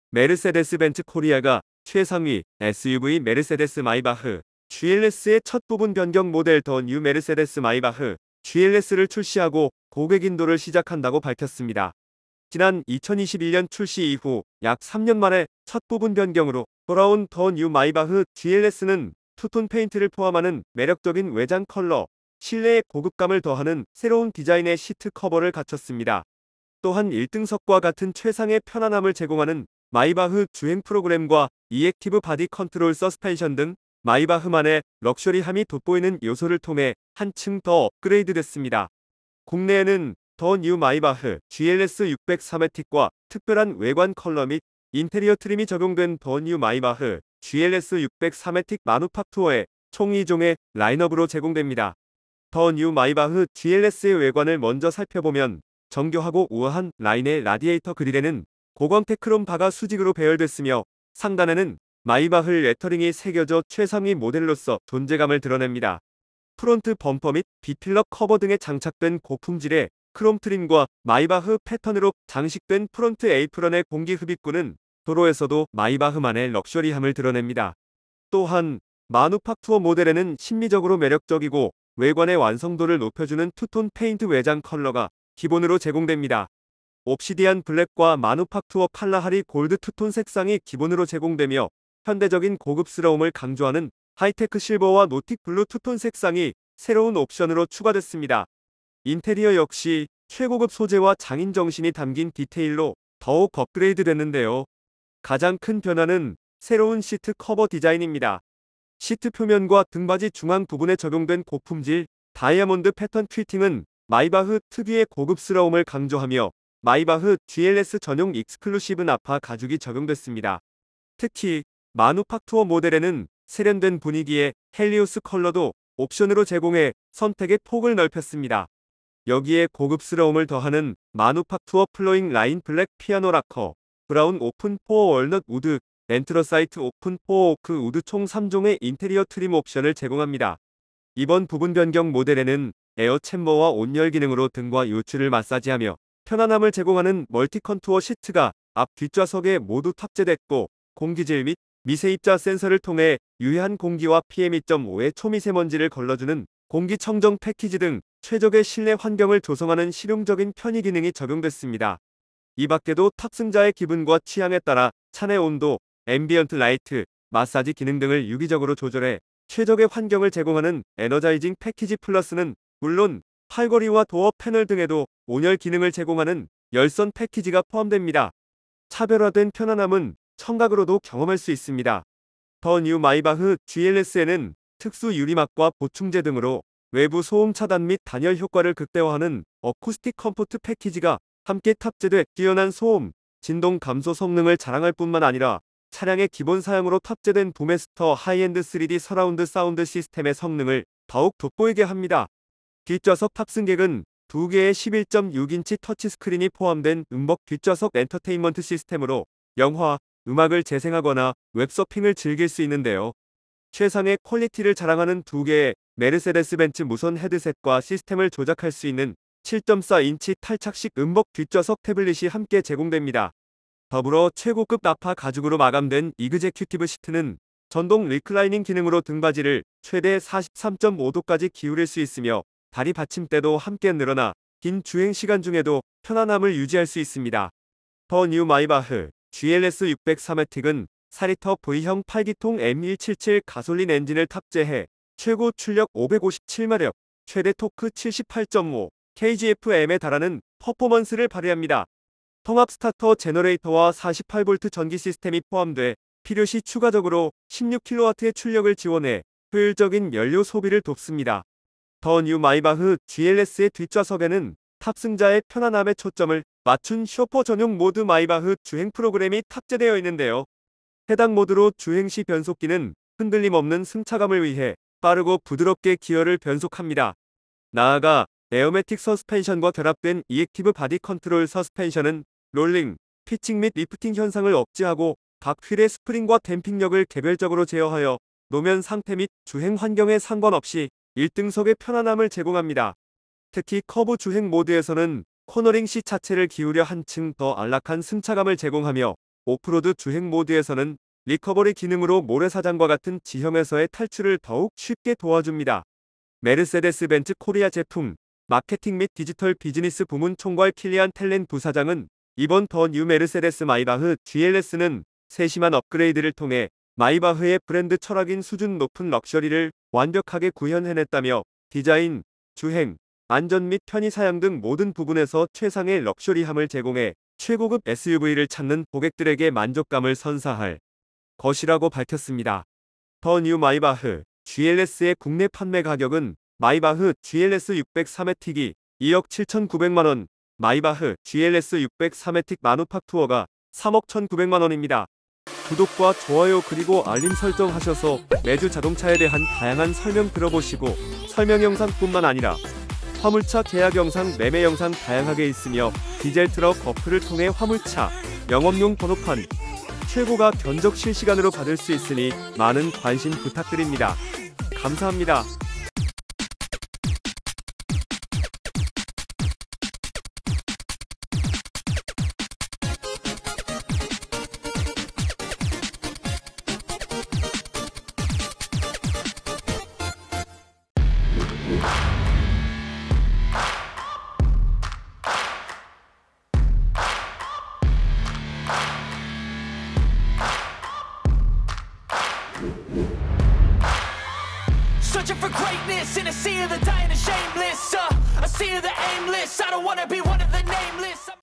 3억원대 최상위 SUV, 메르세데스 마이바흐 GLS 부분변경 국내 출시
11.5톤 12.5톤 14톤 14.5톤 16톤 17톤 18톤 21톤 22.5톤 25톤